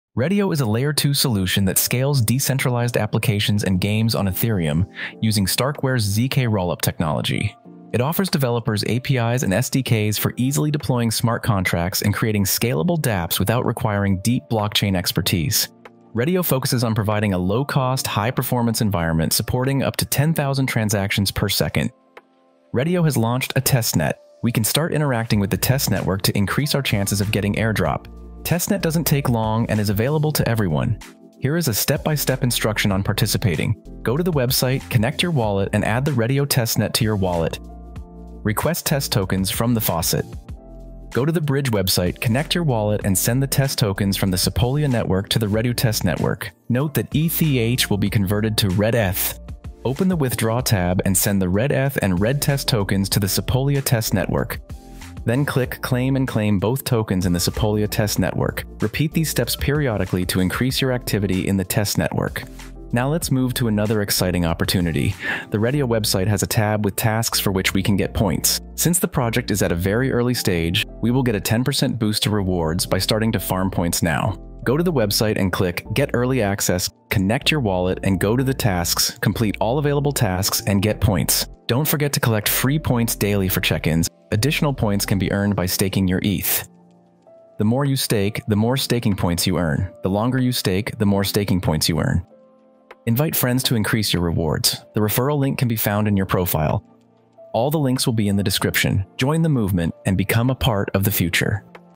Reddio - Early Bird Full Testnet Guide
0:00 - Introduction: What is Reddio
0:22 - Testnet Guide
1:02 - Points Guide